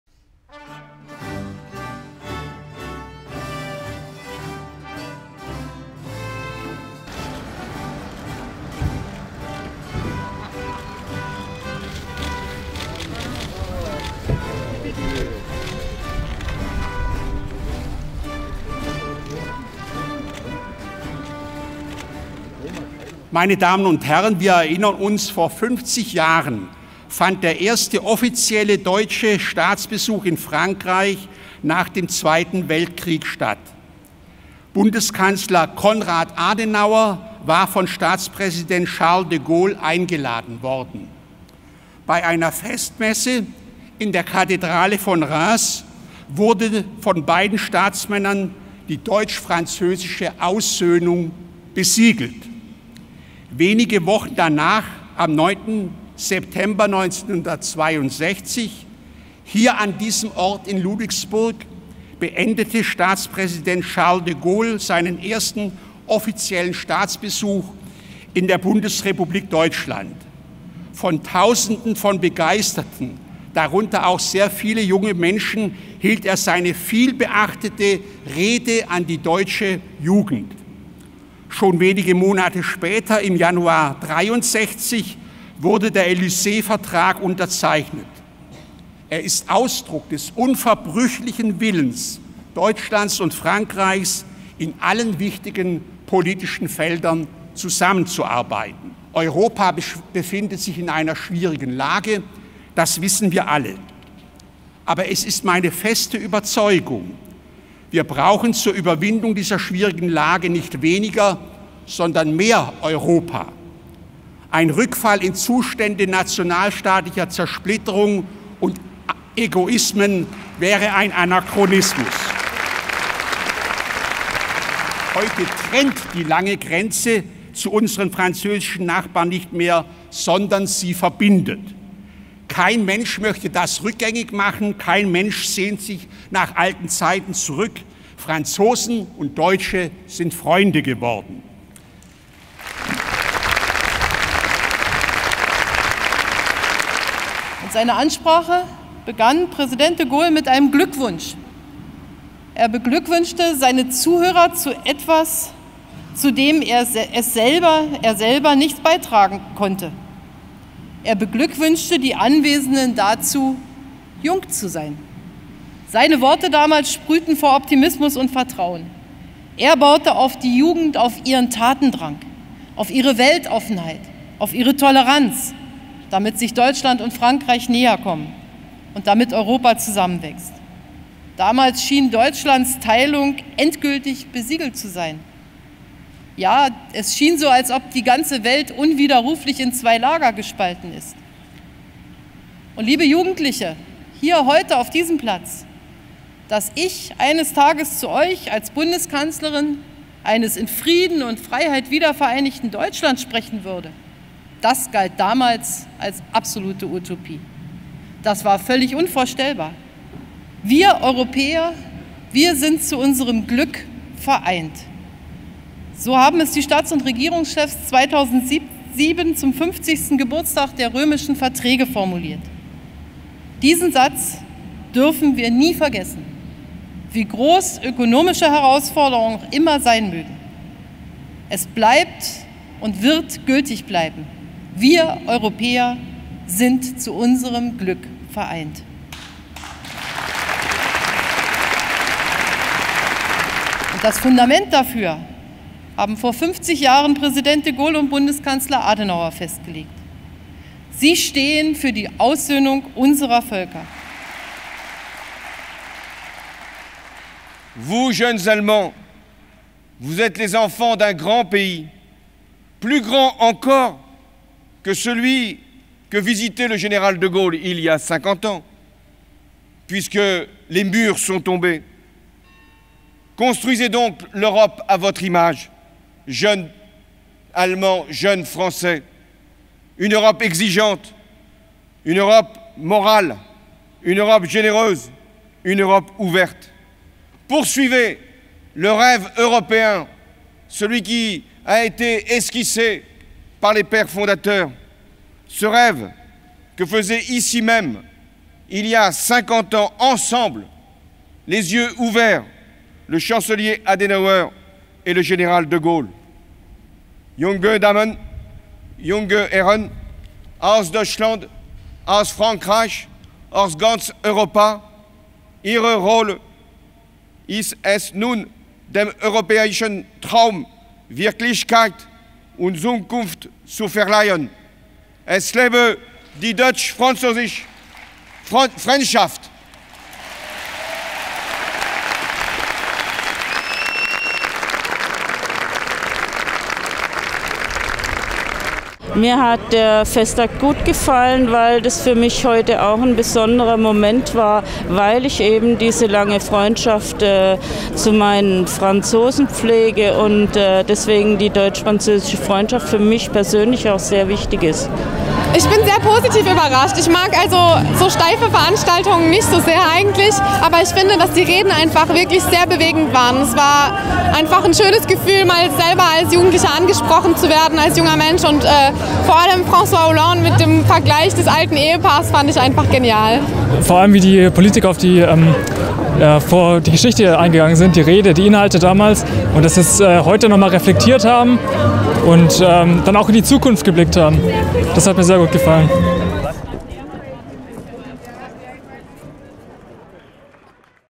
Festakt in Ludwigsburg zum 50-jährigen Bestehen der deutsch-französischen Freundschaft
"Europa befindet sich in einer schwierigen Situation. Aber es ist meine feste Überzeugung: Wir brauchen zur Überwindung dieser schwierigen Lage nicht weniger, sondern mehr Europa", sagte Ministerpräsident Winfried Kretschmann beim Festakt zum 50. Jahrestag der Rede Charles de Gaulles an die deutsche Jugend im Schloss Ludwigsburg. An dem Festakt nahmen Bundeskanzlerin Angela Merkel und der französische Präsident François Hollande teil.